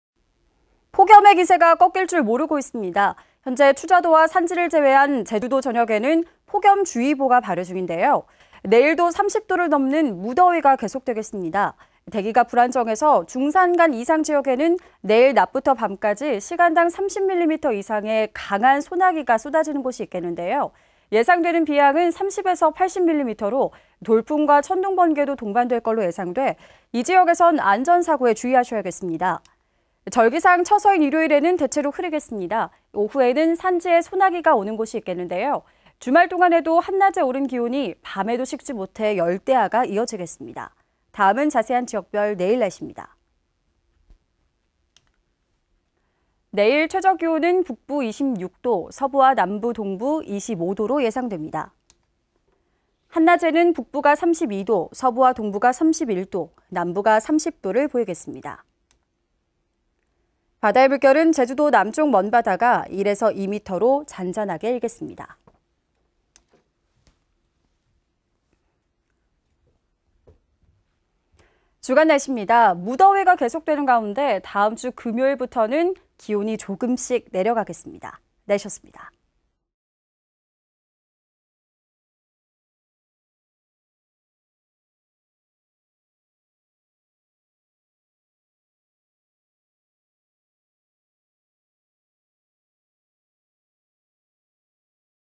무더위 지속 내일 산지 소나기 예상강수량 30~80mm 김은지
CG1. 토요일 날씨

 폭염의 기세가 꺾일 줄 모르고 있습니다.
현재 추자도와 산지를 제외한
제주도 전역에는 폭염주의보가 발효 중인데요.

 내일도 30도를 넘는 무더위가 계속되겠습니다.

 대기가 불안정해 중산간 이상 지역에는
내일 낮부터 밤까지 시간당 30mm 이상의
강한 소나기가 쏟아지는 곳이 있겠는데요,

 예상되는 비의 양은 30에서 80mm로,
돌풍과 천둥번개도 동반할 것으로 예상돼
이 지역에서는 안전사고에 주의하셔야겠습니다.

CG2. 일요일 날씨

 절기상 처서인 일요일에는
대체로 흐리겠습니다.

 오후, 산지에는
소나기가 오는 곳이 있겠는데요,

 주말동안에도 한낮에 오른 기온이
밤에도 식지 못해 열대야가 이어지겠습니다.

 다음은 자세한 지역별 내일 날씨입니다.

(현재)
 내일 최저기온은 북부 26도,
서부와 남부, 동부 25도로 예상됩니다.

(낮 최고기온)
 한낮에는 북부가 32도,
서부와 동부가 31도,
남부가 30도를 보이겠습니다.

(해상)
 바다의 물결은
제주도남쪽먼바다가 1에서 2미터로
잔잔하게 일겠습니다.

(주간 날씨)
 주간날씨입니다.
 무더위가 계속 되는 가운데
다음 주 금요일부터 기온이 조금씩
내려가겠습니다.